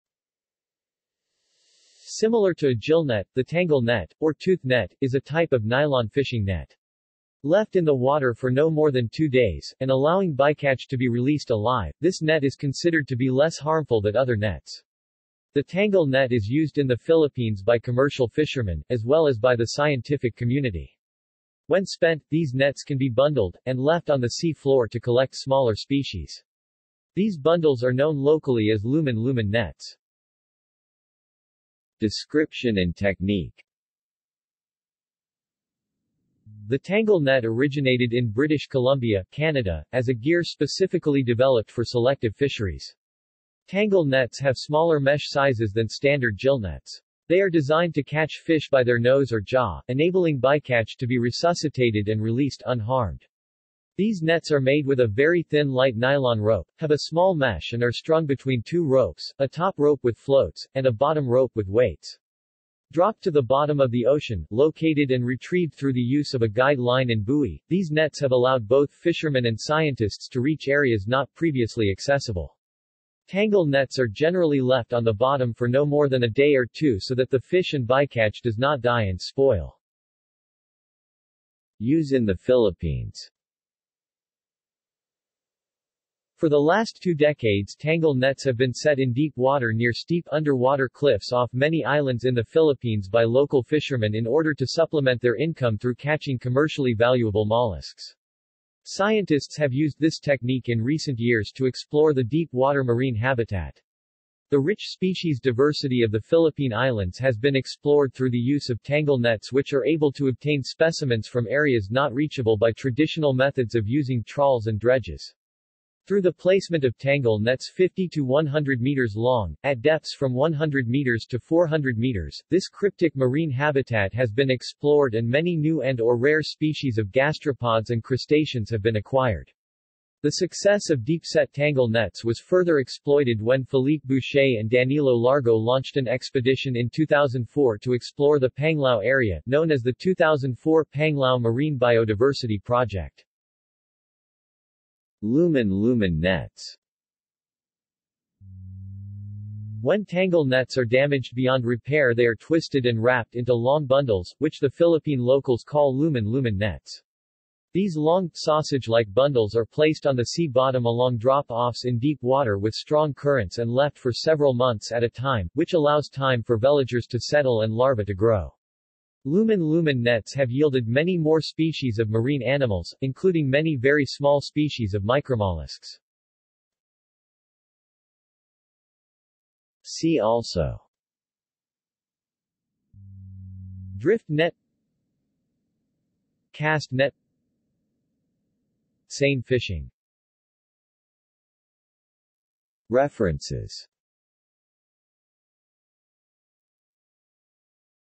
Tangle net | Wikipedia audio article
This is an audio version of the Wikipedia Article:
Tangle net

SUMMARY
Similar to a gillnet, the tangle net, or tooth net, is a type of nylon fishing net. Left in the water for no more than two days, and allowing bycatch to be released alive, this net is considered to be less harmful that other nets. The tangle net is used in the Philippines by commercial fishermen, as well as by the scientific community. When spent, these nets can be bundled, and left on the sea floor to collect smaller species. These bundles are known locally as lumen lumen nets.